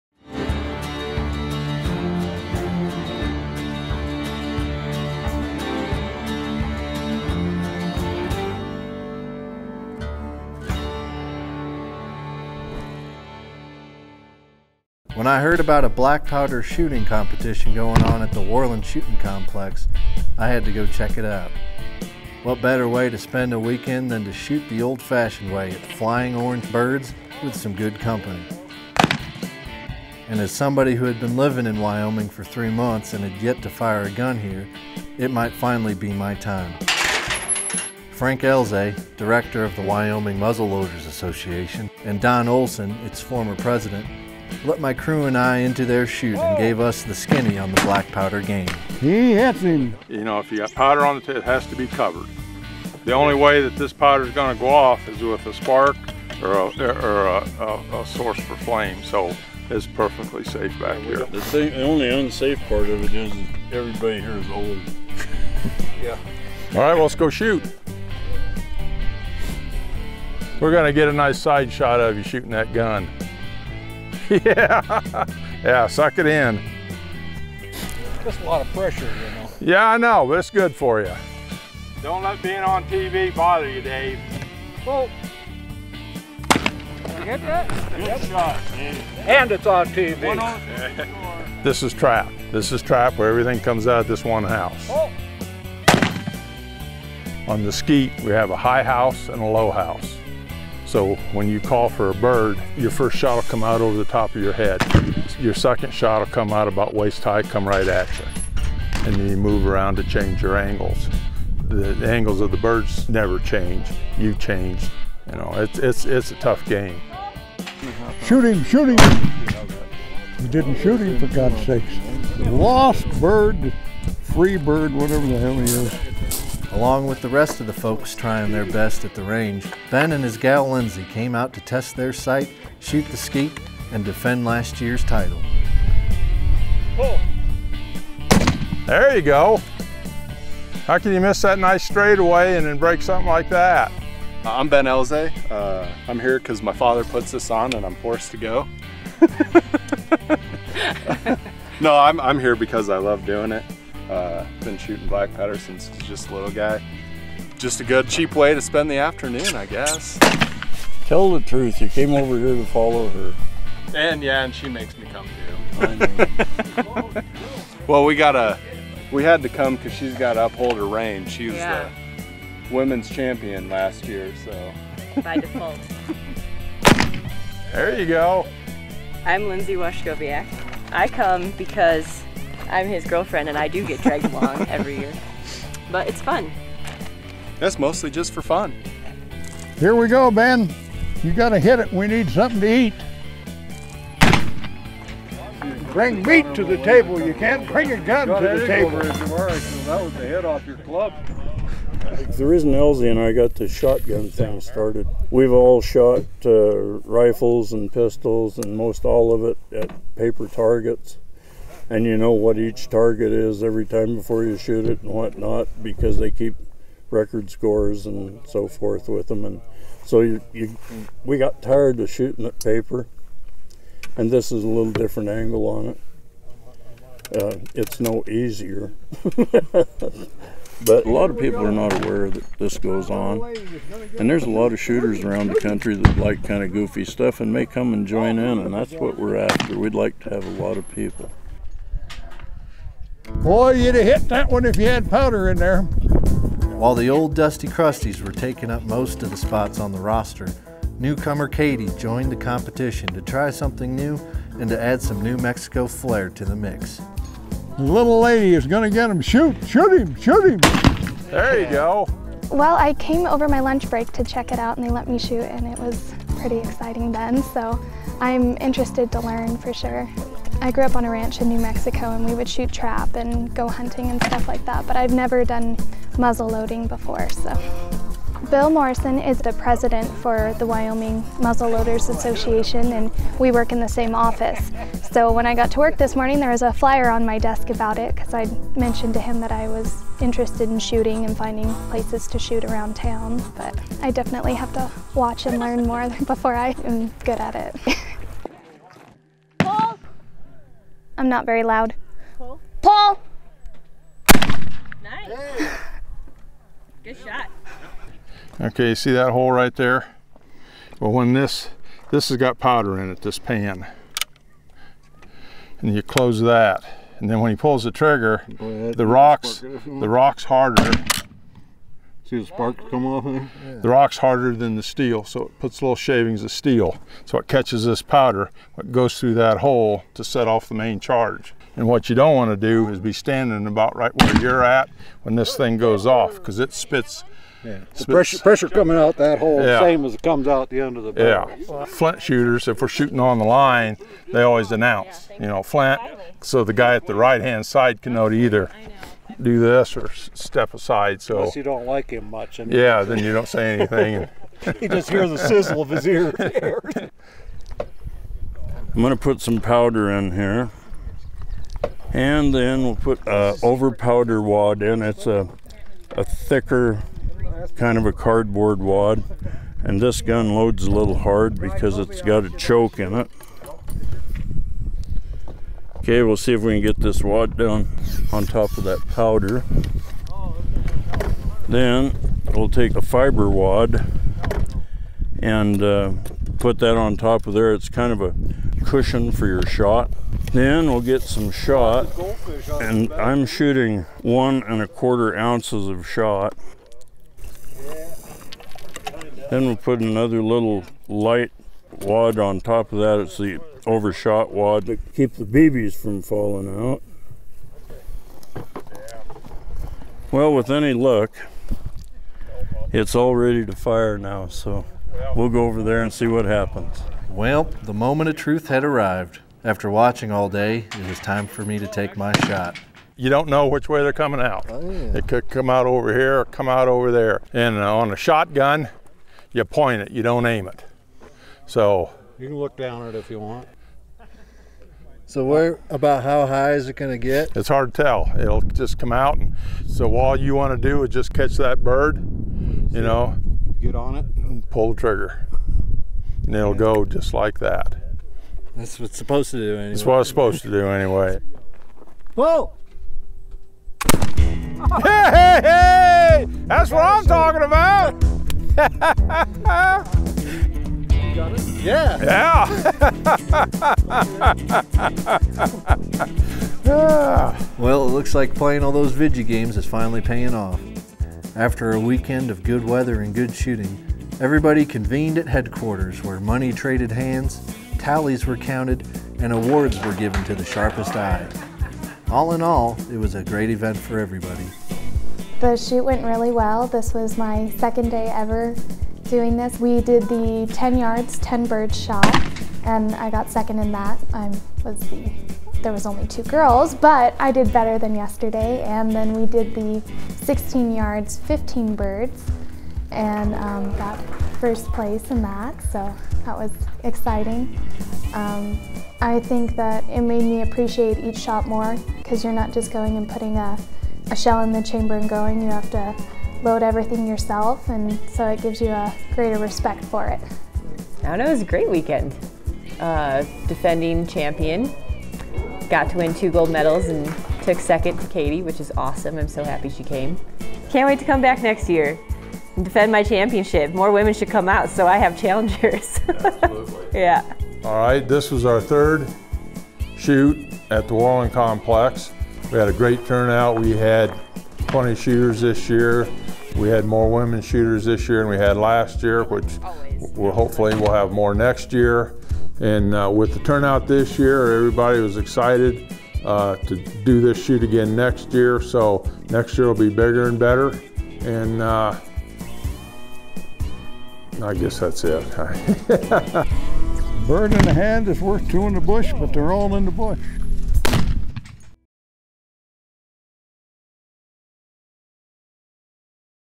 The Muzzle Loaders - Our Wyoming
Black powder skeet and trap shooting is a thing in Wyoming.